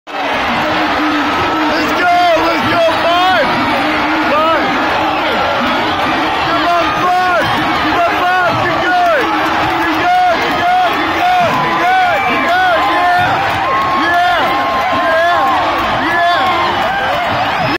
Derby race #5 The 143rd Kentucky Derby
Always Dreaming Wins The 143rd Kentucky Derby